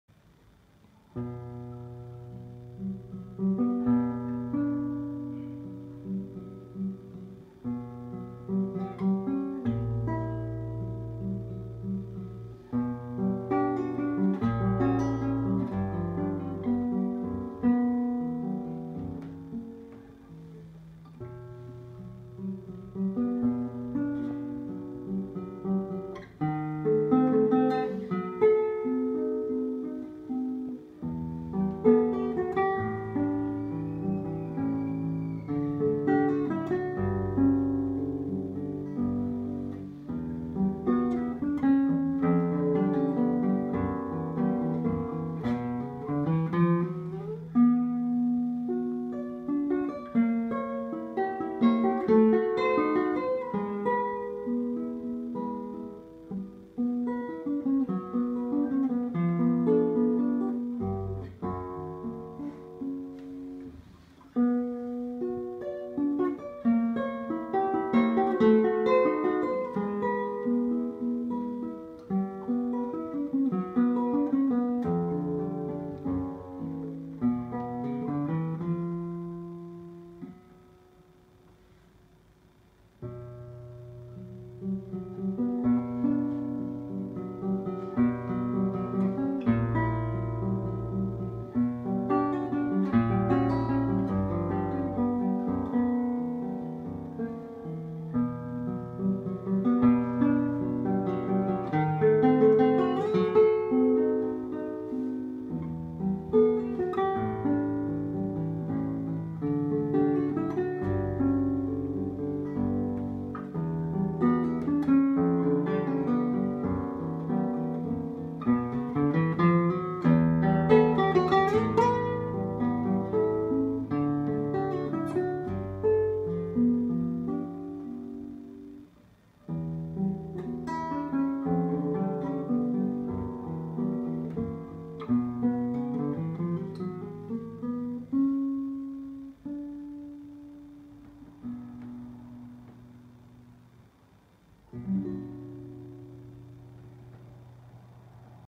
Super seasoned pine classical guitar sound. Part 1
Handmade Pine Guitar Project: how to build your own guitar.
Recorded with IPhone5s.